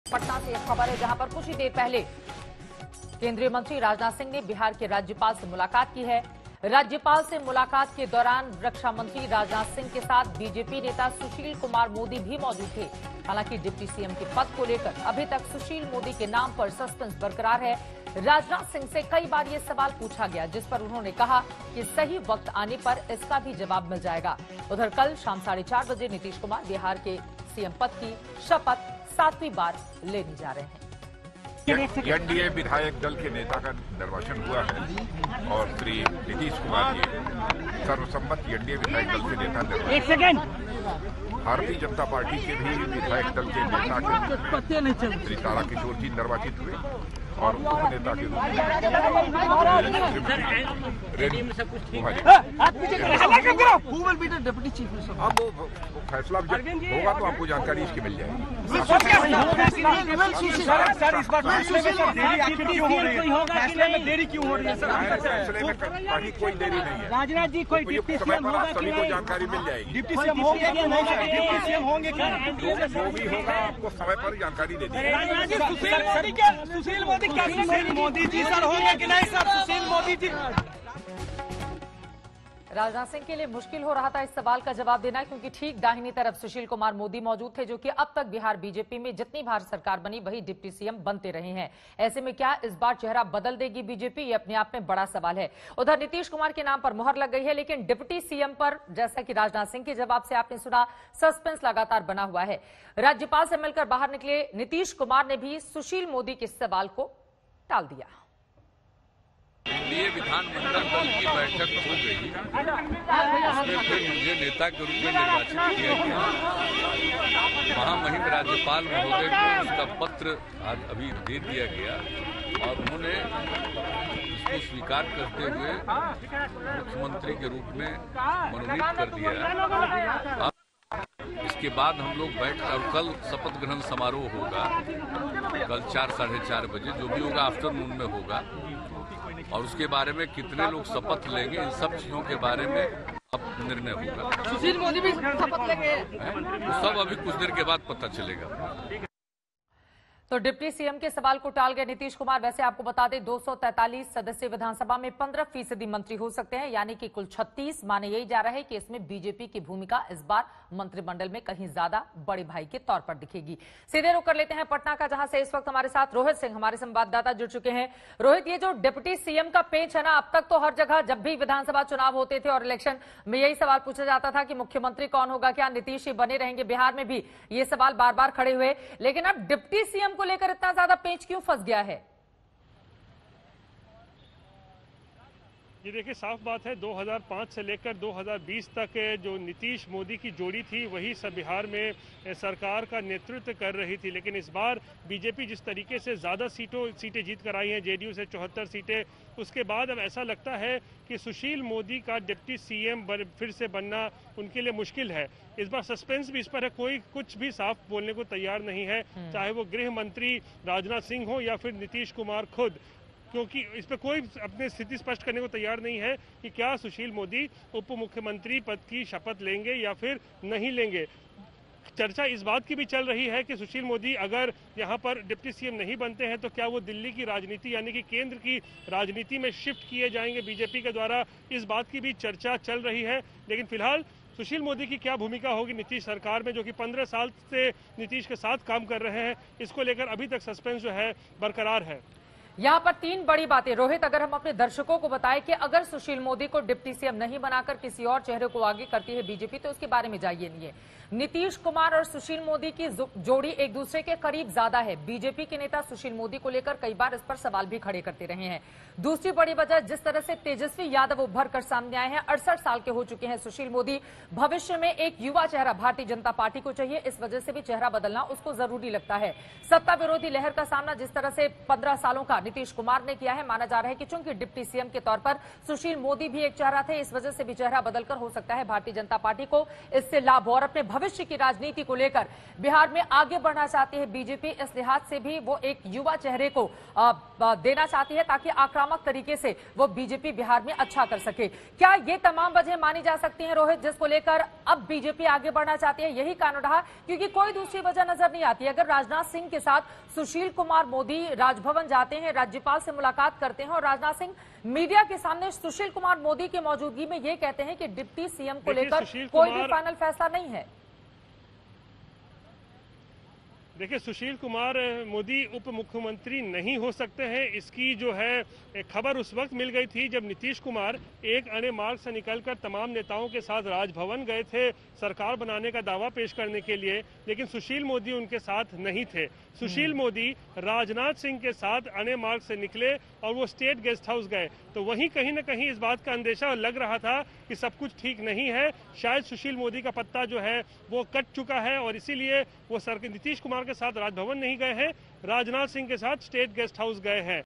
Bihar Govt Formation: Rajnath Singh बोले- समय आने पर बता देंगे कौन बनेगा बिहार का Deputy CM
बिहार में डेप्युटी सीएम यानी उपमुख्यमंत्री कौन होगा, इसको लेकर सस्पेंस बरकरार है। अभी तक ये तय नहीं हो पाया है कि इस पद पर सुशील मोदी बने रहेंगे या किसी नए चेहरे को ये जिम्मेदारी मिलेगी। पटना स्थित सीएम आवास से बाहर निकले केंद्रीय रक्षा मंत्री राजनाथ सिंह ने मीडिया के सवालों का जवाब देते हुए कहा कि डेप्युटी सीएम का फैसला सही समय पर होगा। बैठकर इसका फैसला किया जाएगा